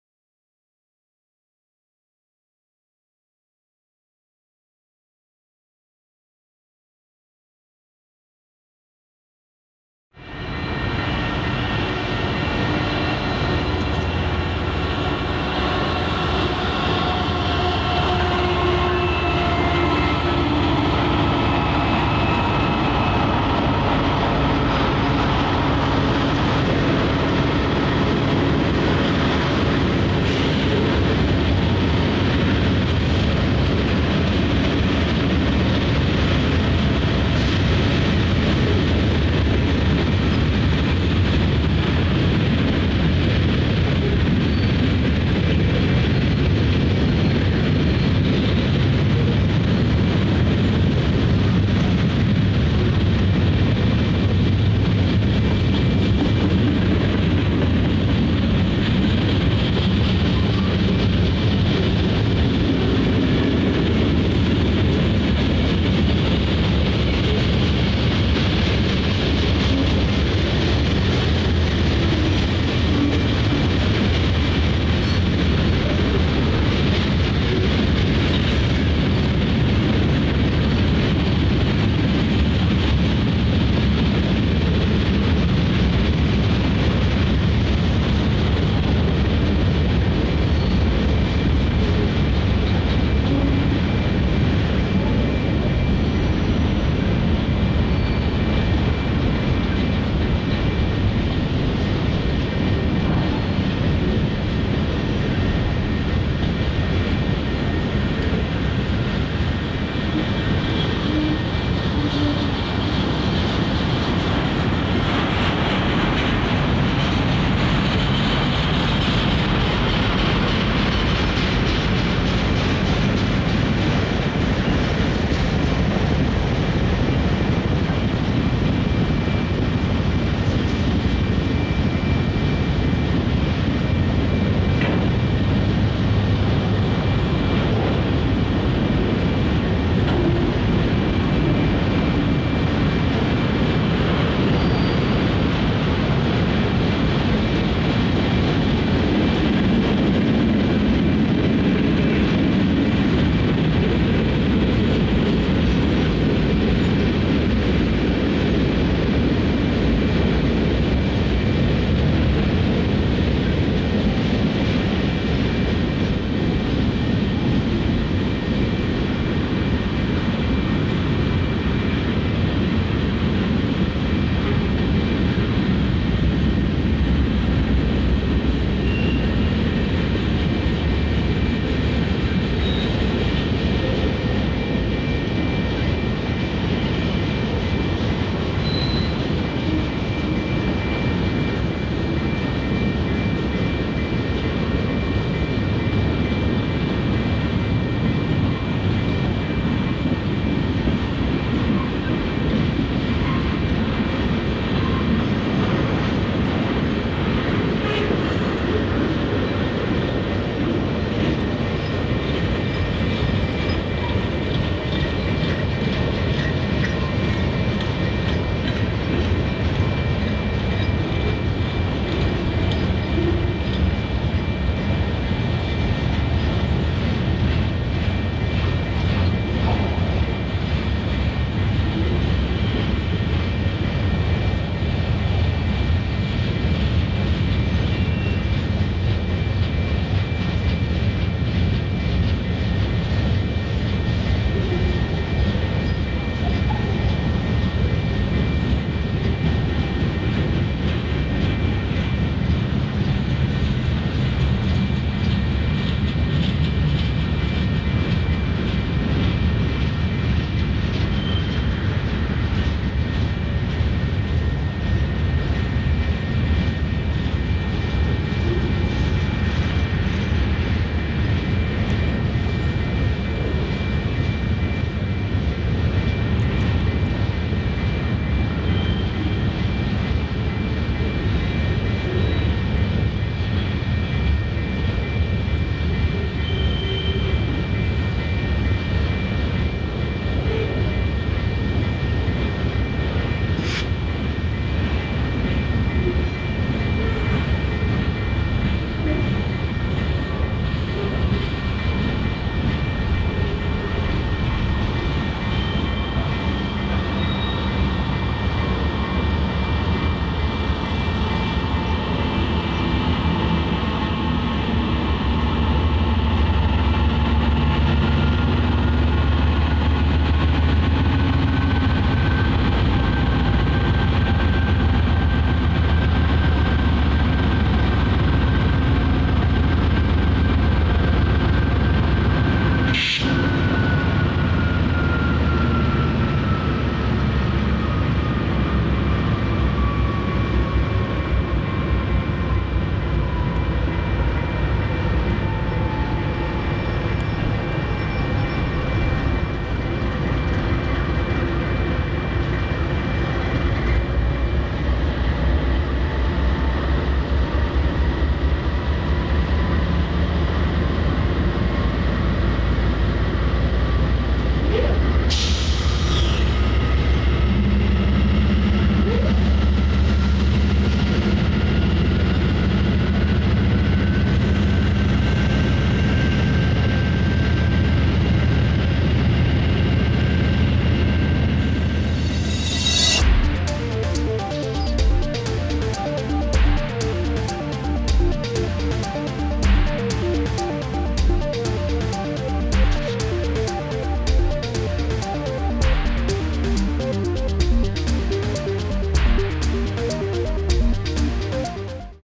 Union Pacific GE - Eastbound Manifest - Cheyenne Wyoming - 6/17/2018
In the Summer of 2018 I had the chance to visit the famed Union Pacific Sherman Hill. This is the stretch of line between Laramie Wyoming and Cheyenne Wyoming and has three different mains across it.

Important Links